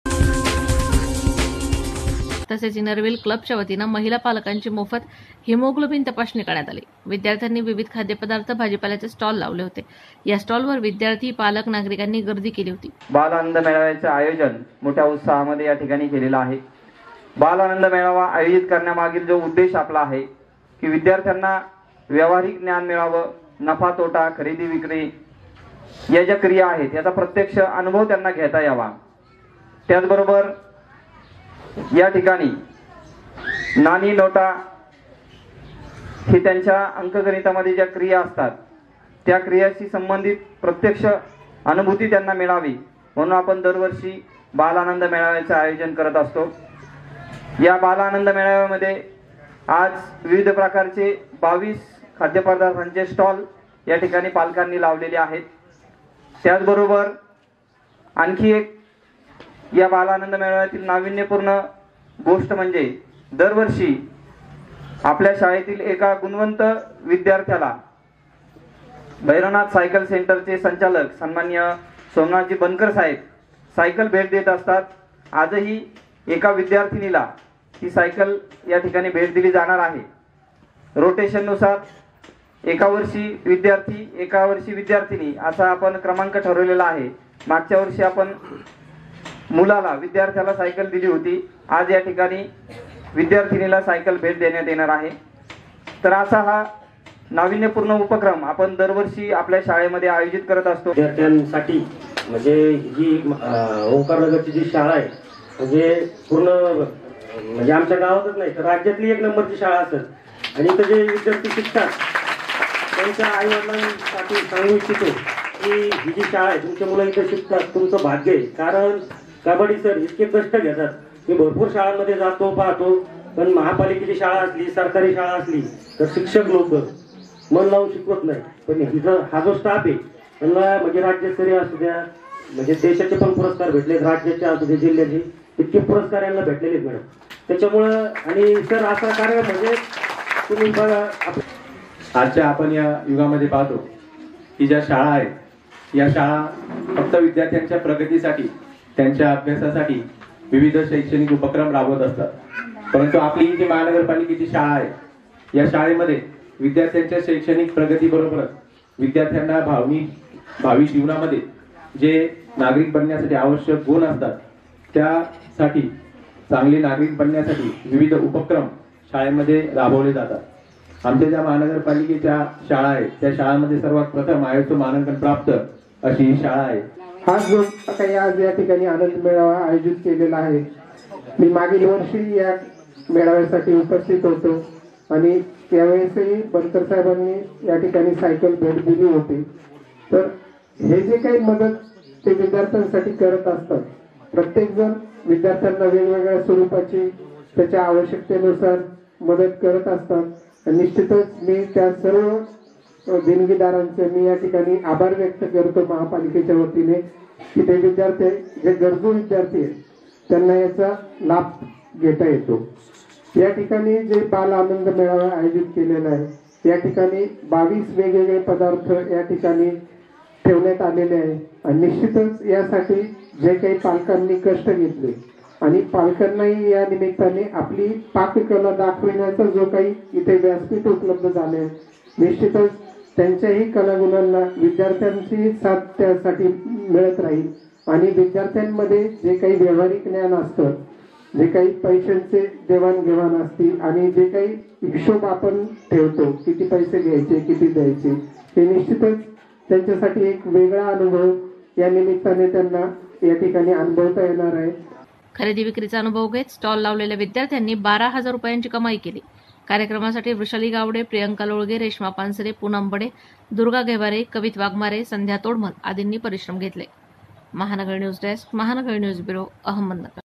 केडगाव ओंकारनगर शाळेत बाल आनंद मेळावा उत्साहात
विद्यार्थ्यांनी गिरवले मार्केटिंगचे धडे, विविध स्टॉल्सवर मोठी गर्दी